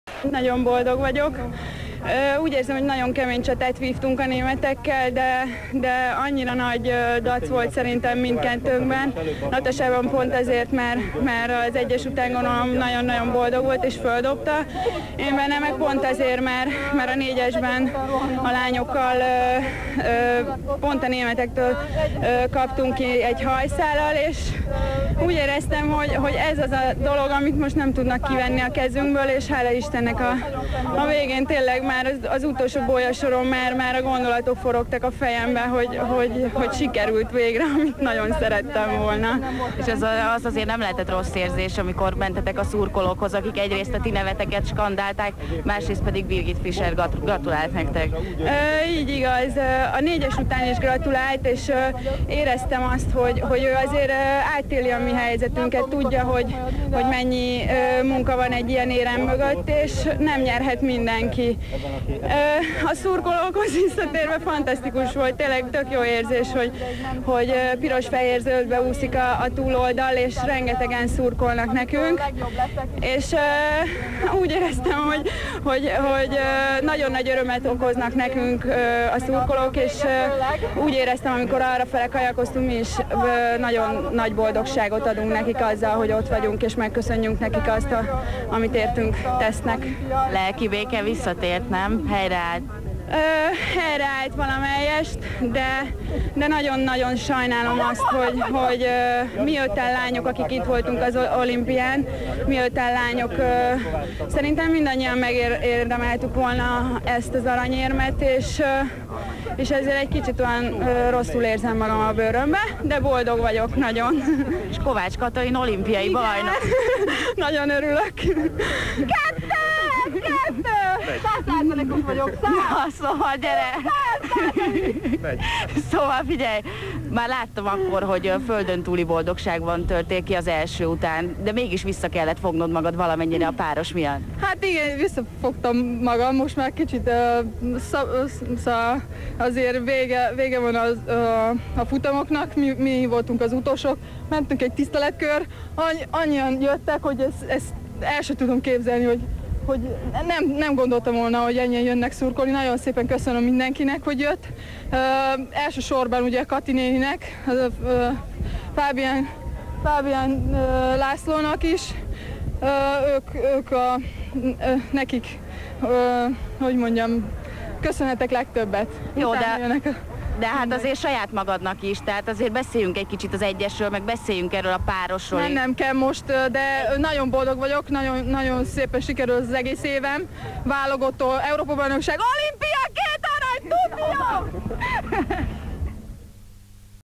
Athén 2004 - Kovács Kati és Janics Natasa vigyorgós interjúi a versenyek után
Az MTV riportjai